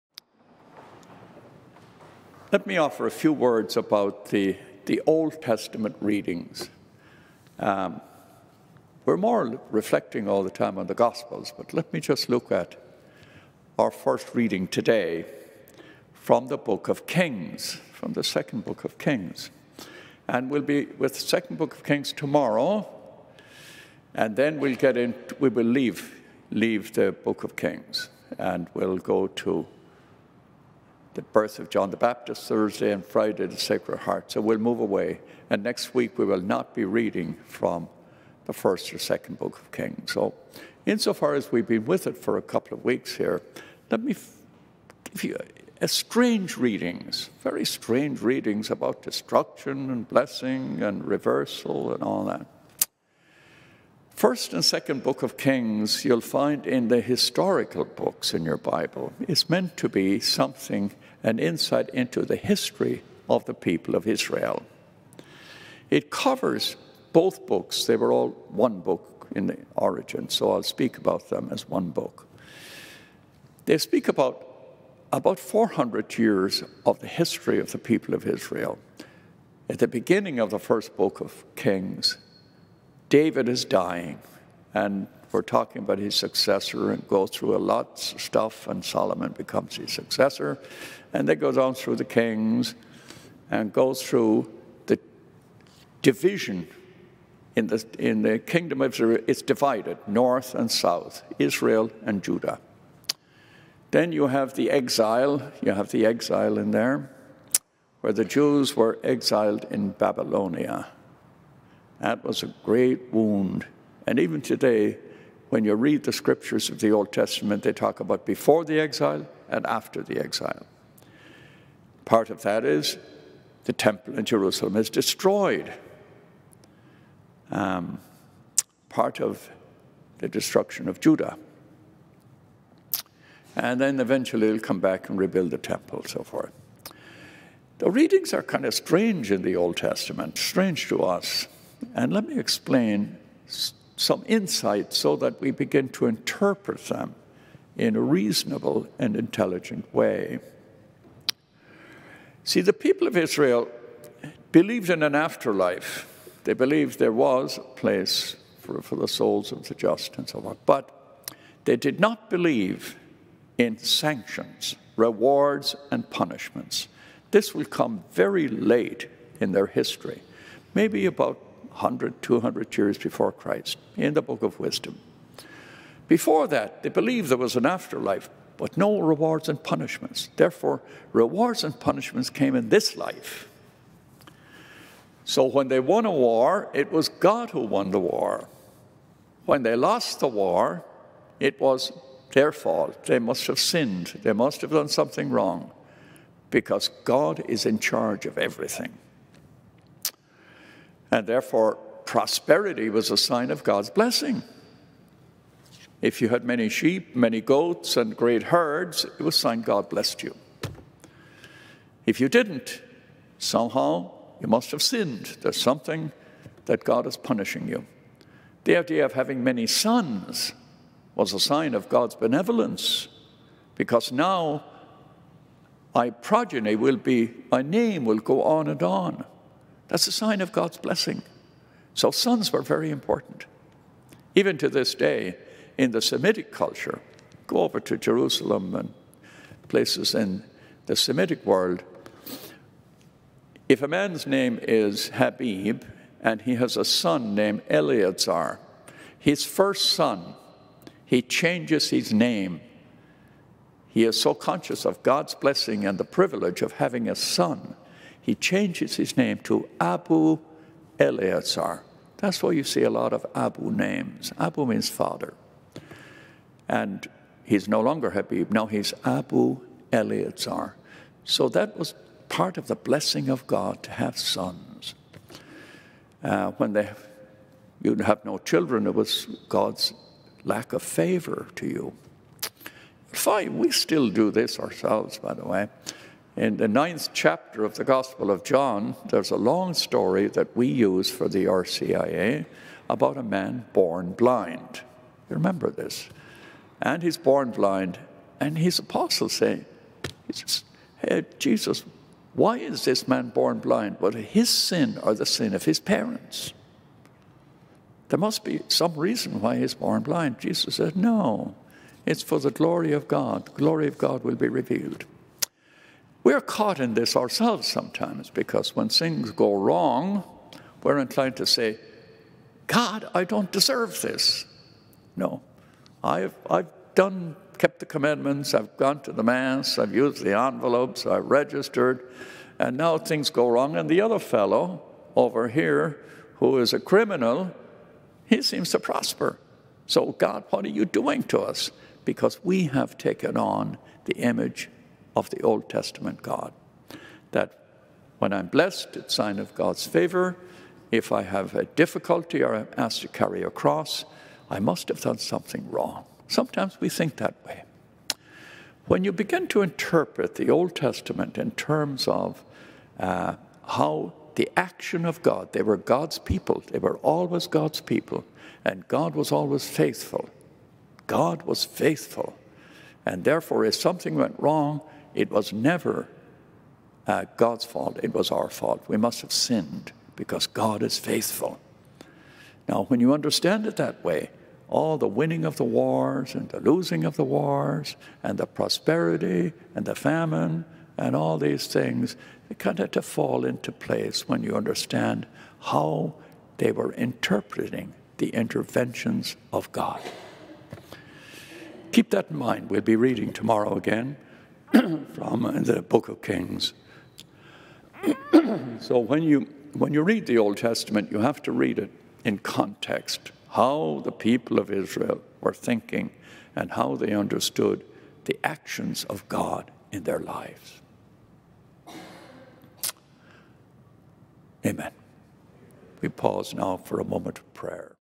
Memorial of Saint Aloysius Gonzaga, June 21, 2022,
Homilist:  Msgr. Clement J. Connolly, Gospel: Matthew 7:6, 12-14, Do to others whatever you would have them do to you.